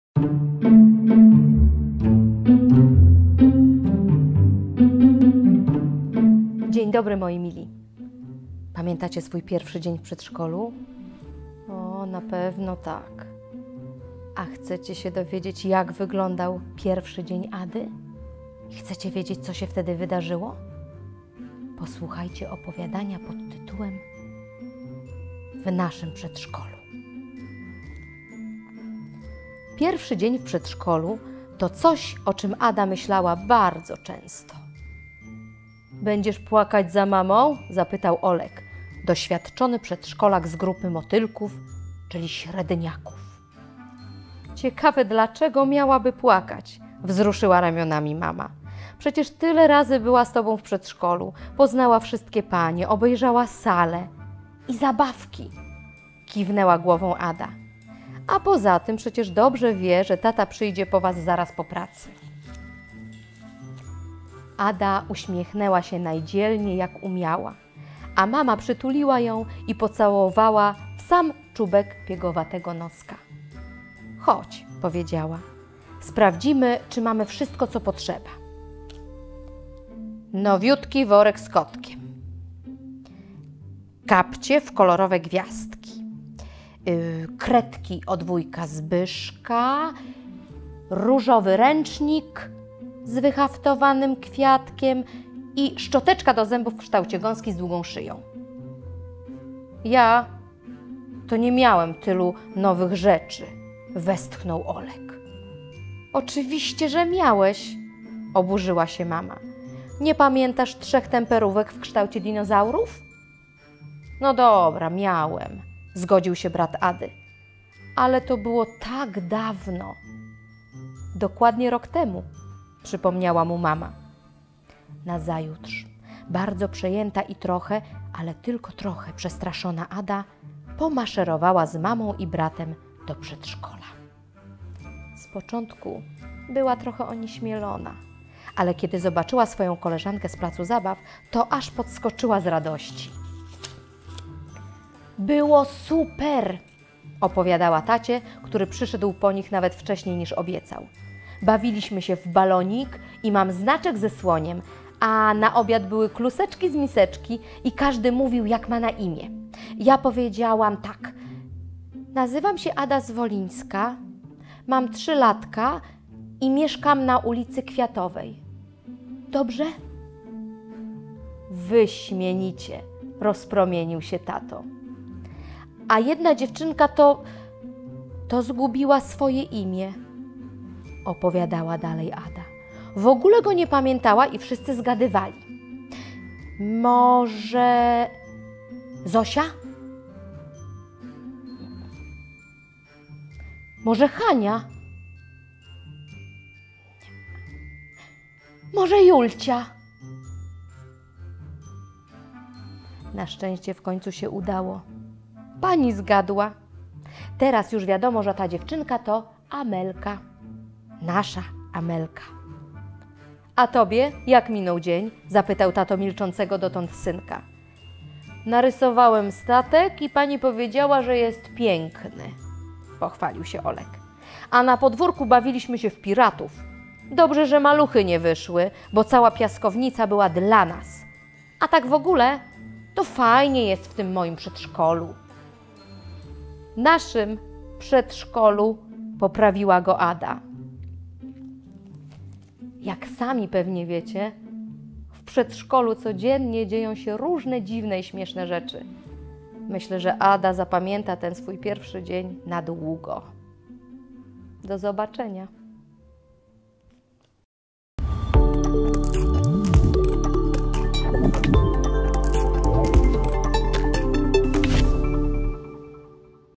Opowiadanie Olek i Ada ,,W naszym przedszkolu'', czyta Monika Filipowicz.
Opowiadanie "W naszym przedszkolu'" autorstwa Ewy Stadmüller.
Przygodę uroczego i rezolutnego rodzeństwa - Olka i Ady, czyta Monika Filipowicz.